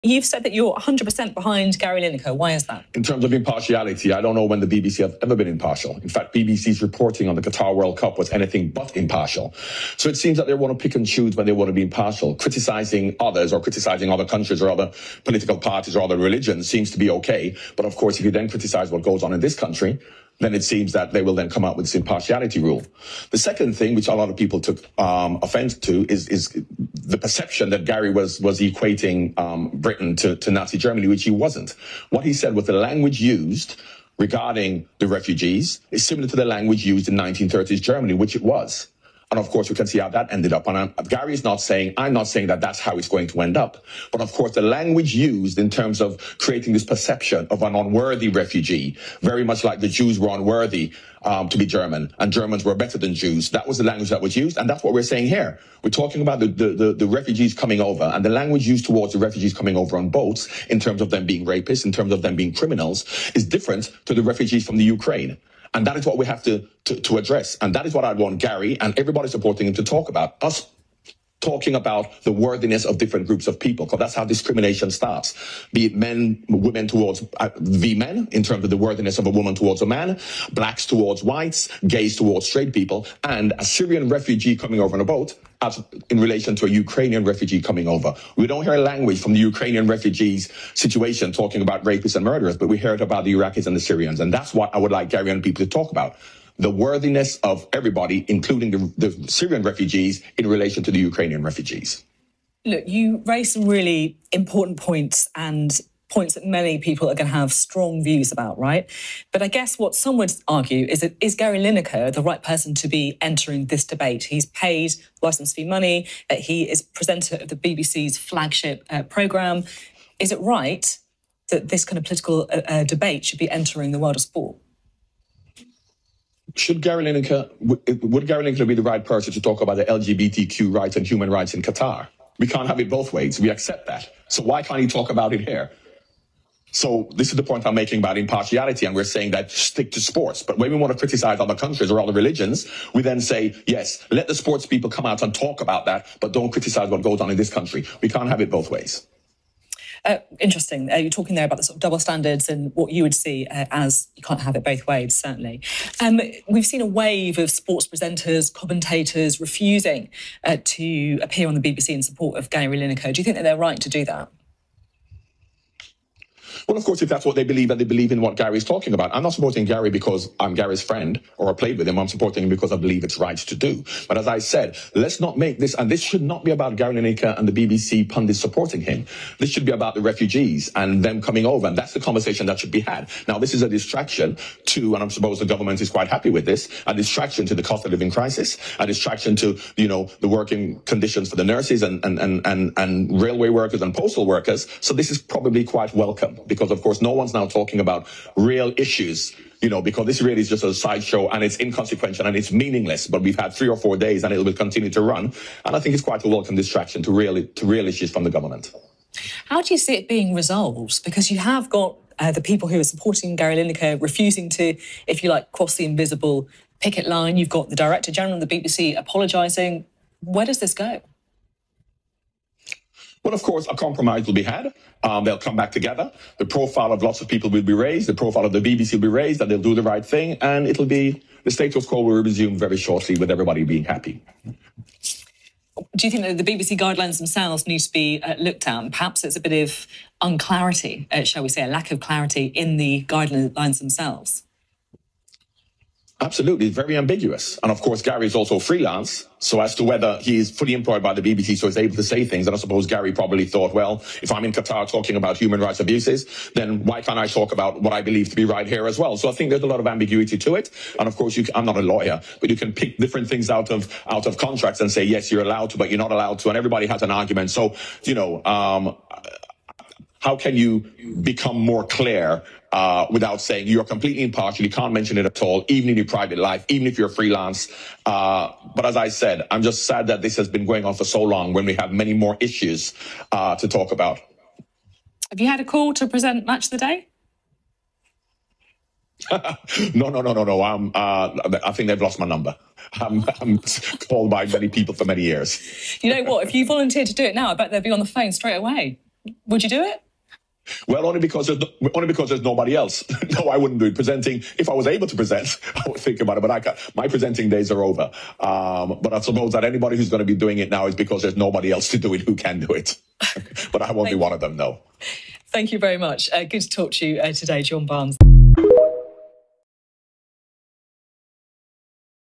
Gary Lineker Row: Football Legend John Barnes Exposes BBC & Governments Hypocrisy/Double Standards
John Barnes, a Football/Soccer Legend for both England and Liverpool brilliantly exposes the sheer hypocrisy and double standards of the Government and BBC over comments recently made by Gary Lineker.

He also quite rightly explicates how this is all a smoke screen to divert the public’s attention from the real issues affecting the public, from the Cost of Living Crisis, to rising energy bills and an erosion of the Rule of Law.